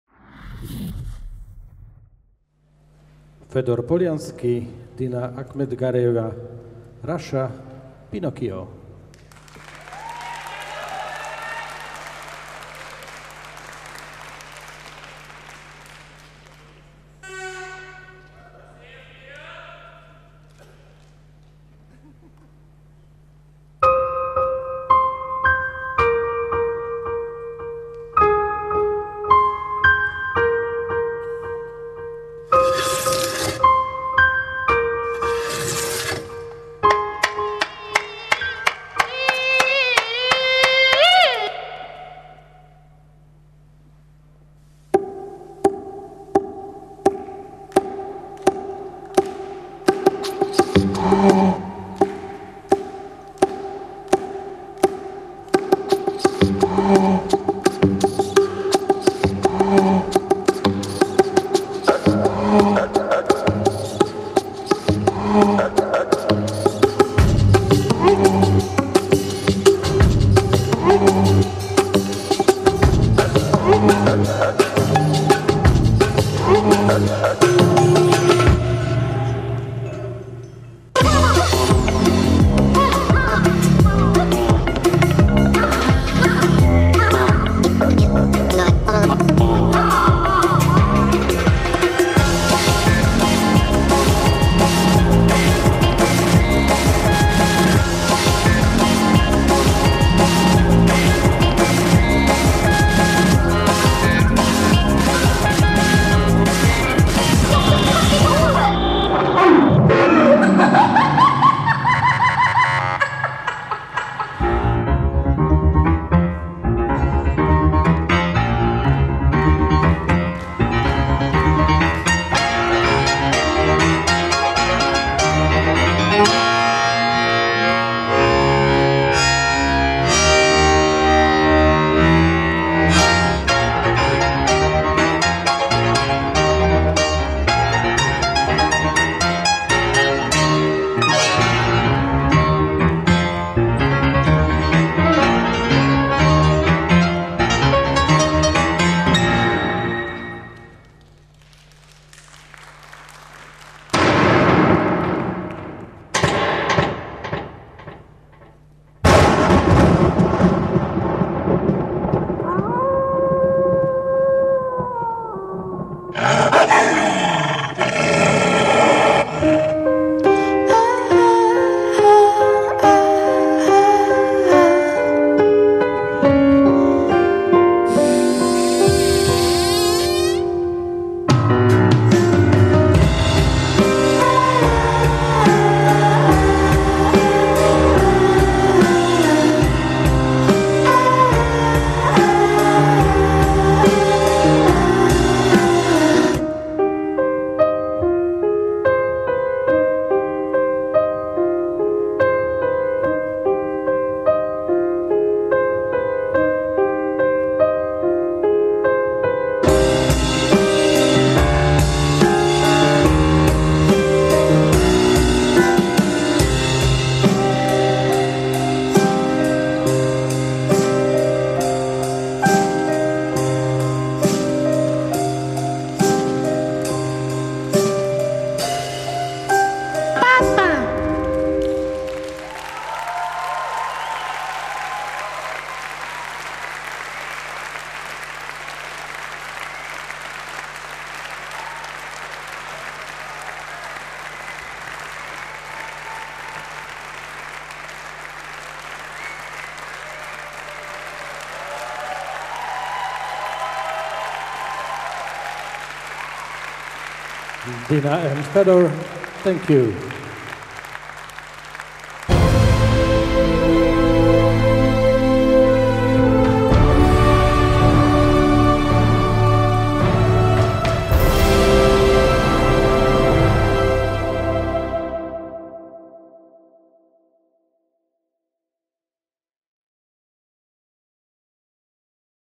Polianski - Akhmetgareeva, RUS | 2018 WDSF WC Showdance Latin Final | DanceSport Total
Fedor Poliansky - Dina Akhmetgareeva, RUS | 2018 WDSF WC Showdance Latin Final | DanceSport Total

Fedor Poliansky_Dina Akhmetgareeva, RUS, dance the Final of the 2018 WDSF World Championship ShowDance Latin on 22 September in Bratislava, SVK.